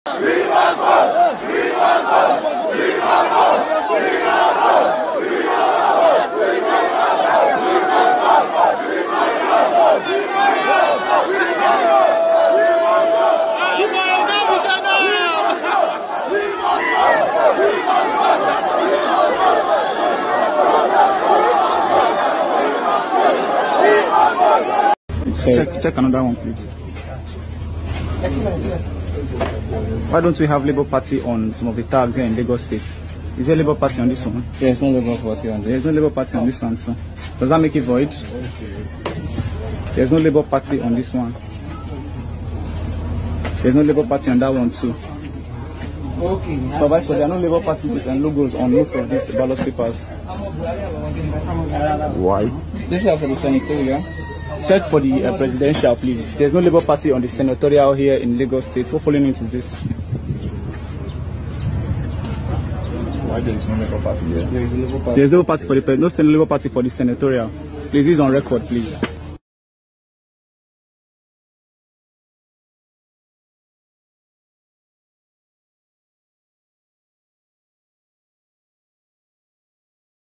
LIVE: What is Currently Happening at Lugbe Abuja Polling Unit
LIVE from lugbe axis of Abuja where votes are casted for the 2023 Presidential Election